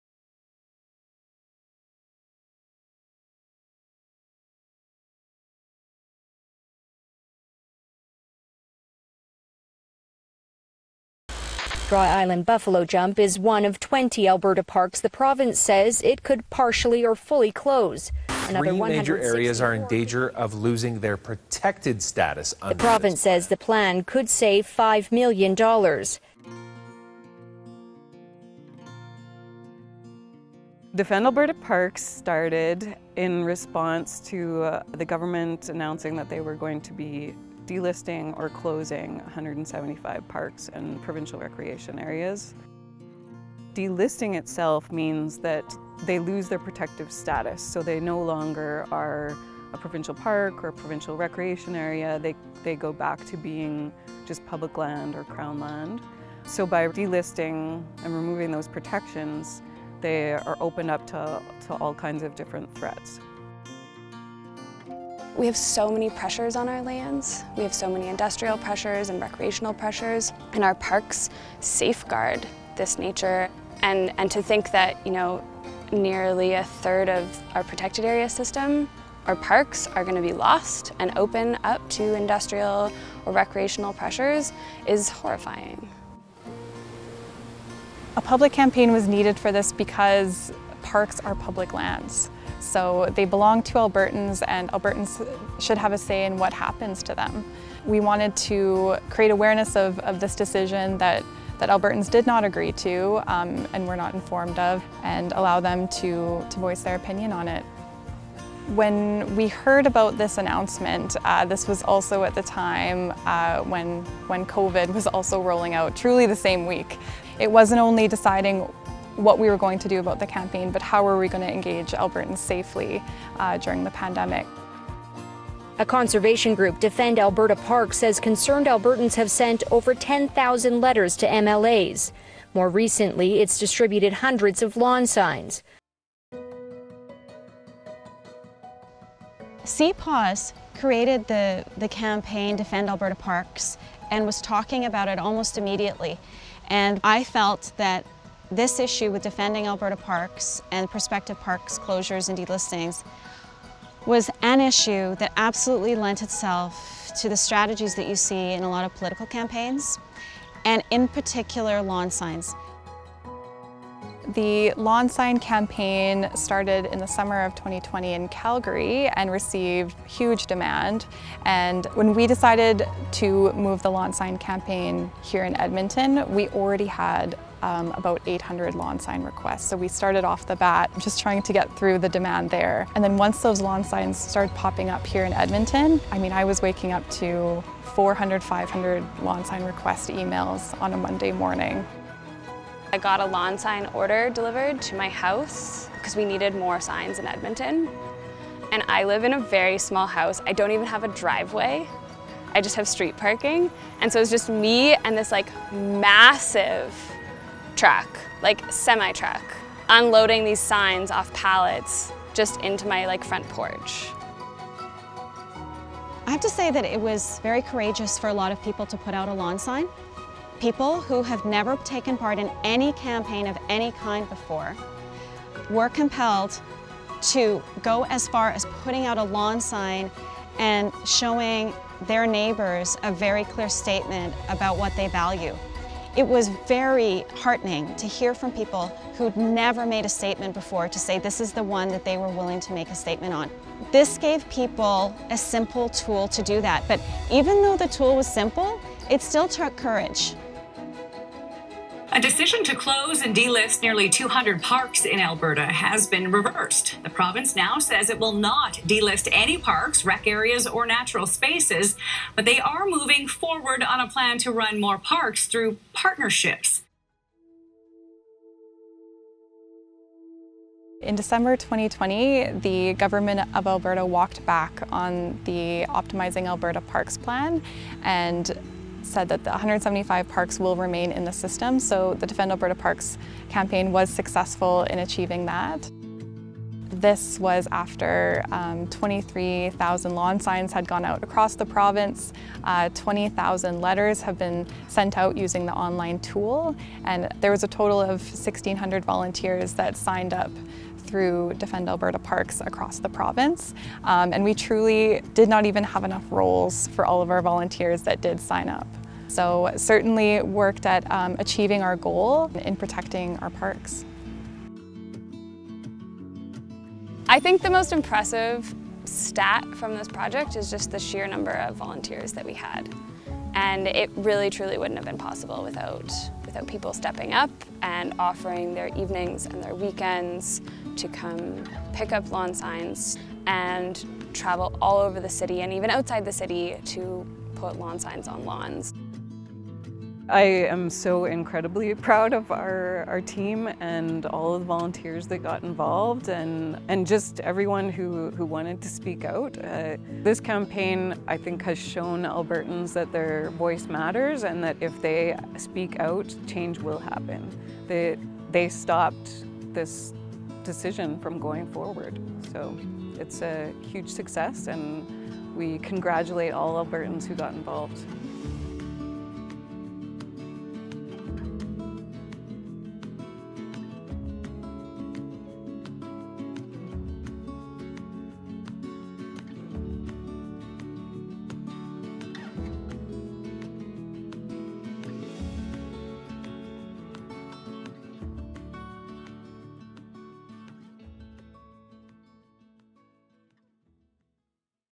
Defend Alberta Parks: Emerald Documentary Series Season Three
Defend Alberta Parks, recipient in the Public Outreach & Engagement category of the 30th Annual Emerald Awards, helped save 175 provincial parks from being removed from the Alberta Parks system.

Learn more about the 30th Annual Emerald Award recipients at emeraldfoundation.ca/30th-annual-emerald-awards/